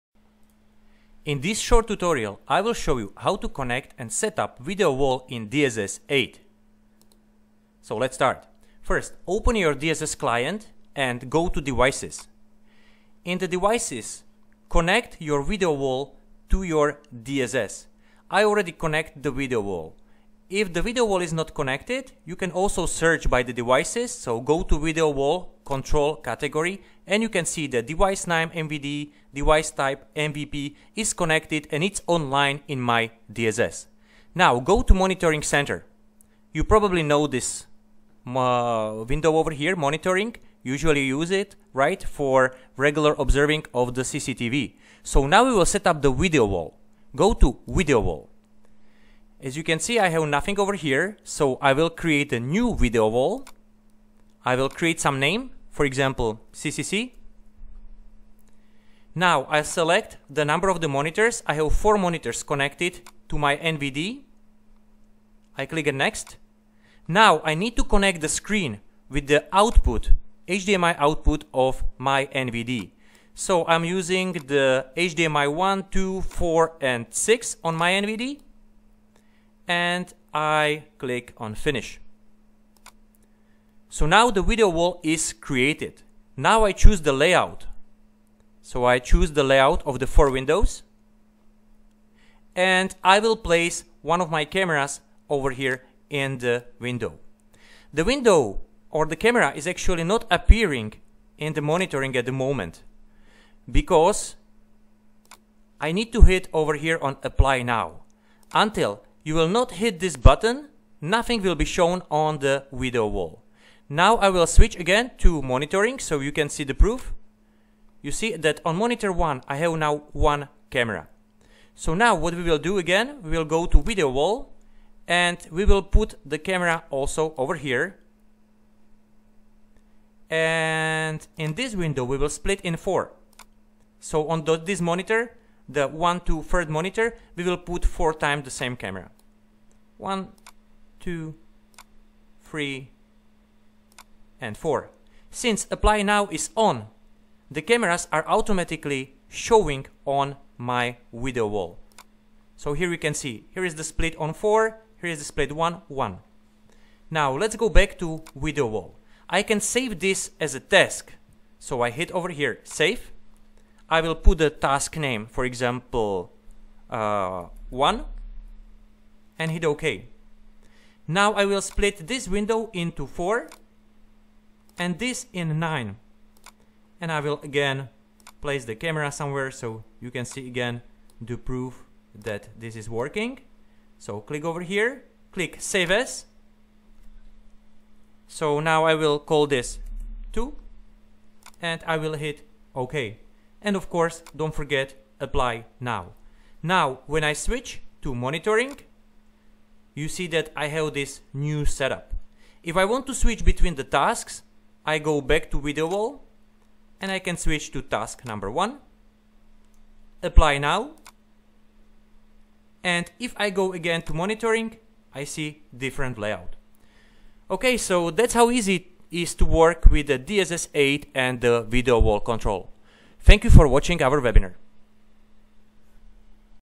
DSS V8 How to setup Video Wall
In this tutorial I am showing how to setup and work with Vide Wall